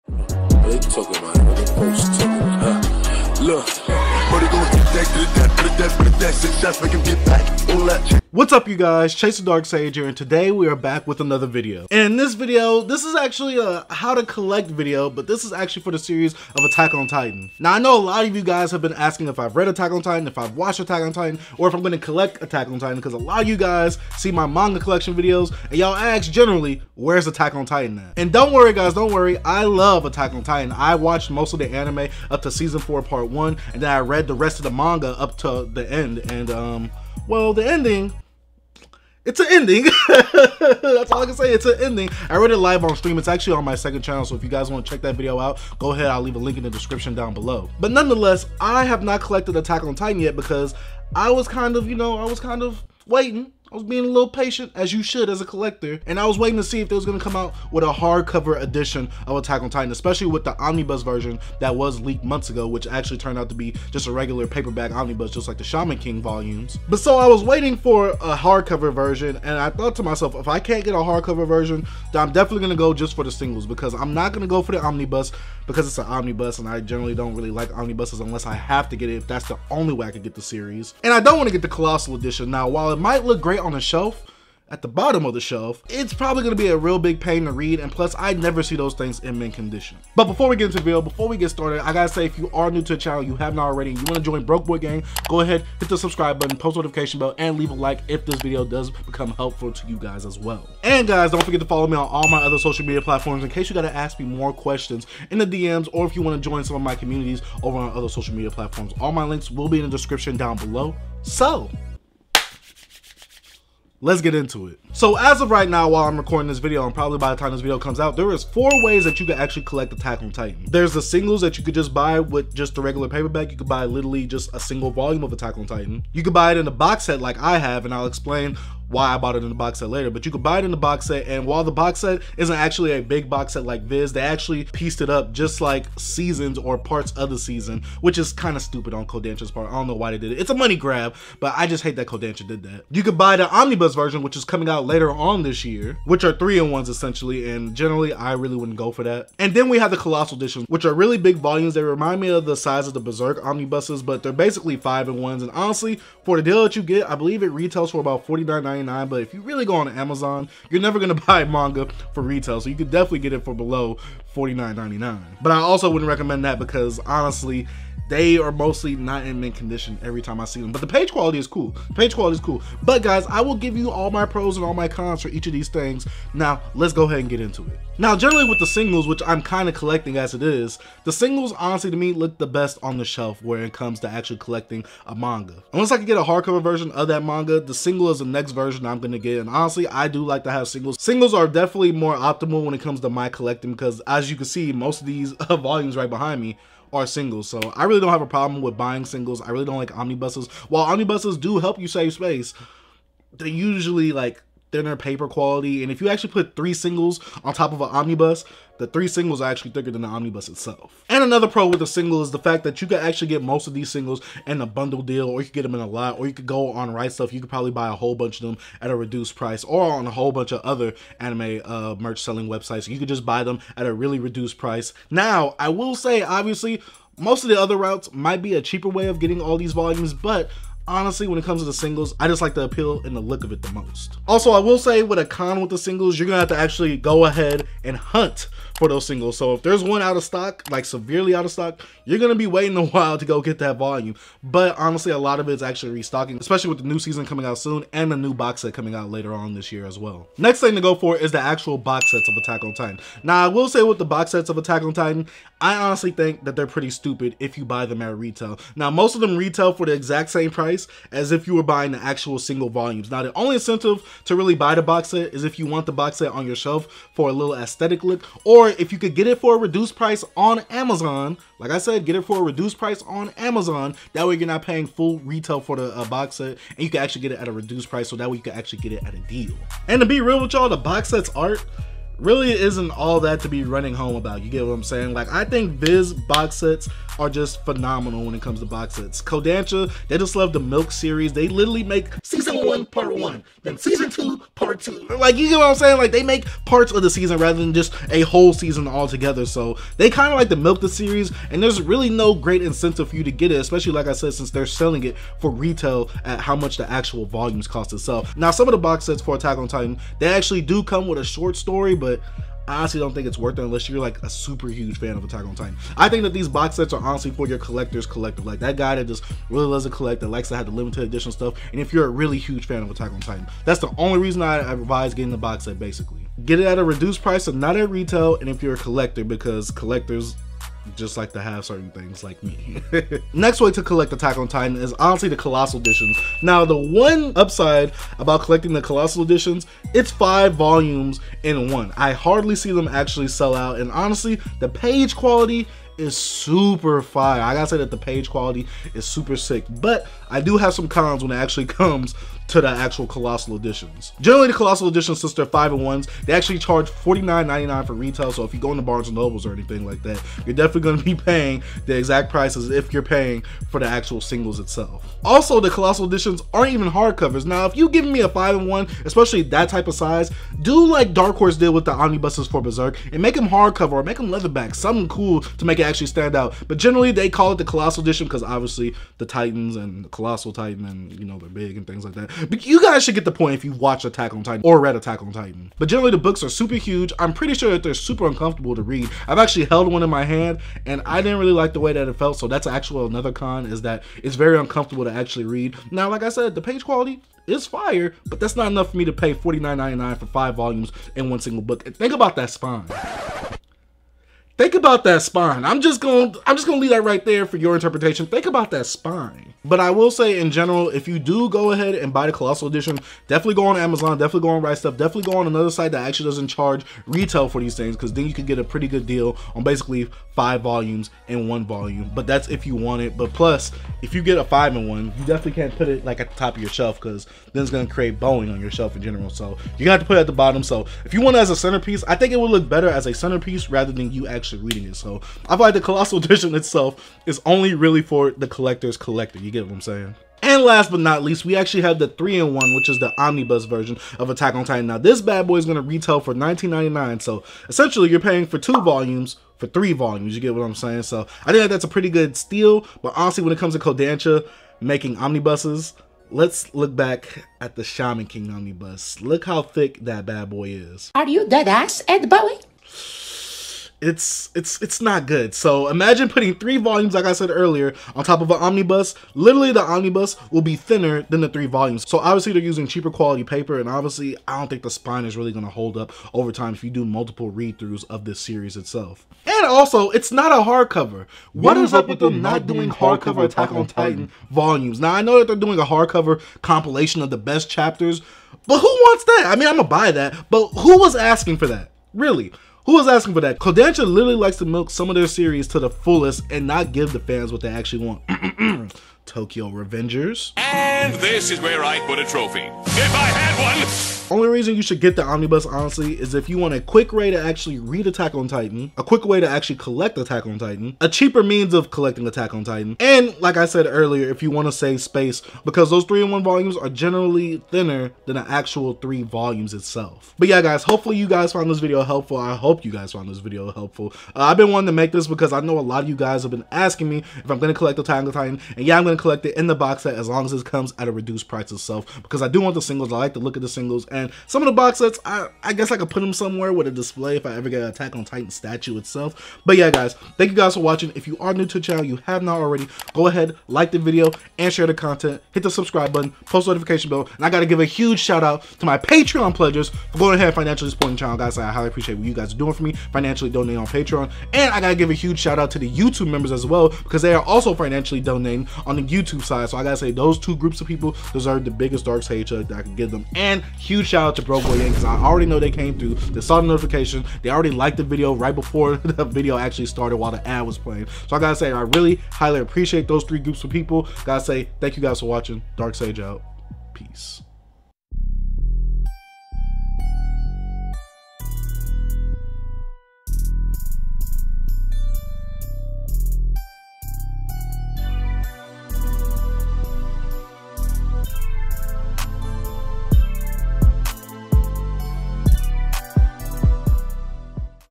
The Best Way to Collect the Attack on Titan Manga!
Welcome to my video where I tell you the pros, cons and best way to collect the Attack on Titan manga!

Timestamps:
0:00 - Intro/Timestamps
1:54 - Brief Description of AoT Manga Versions
2:57 - Pros and Cons to each version
8:40 - Shameless Plugs/Outro

My Discord is CLOSED for now and will have random/limited invites dropped in my community tab.

All opinions in this video are of mine and not anyone else or company's I work with.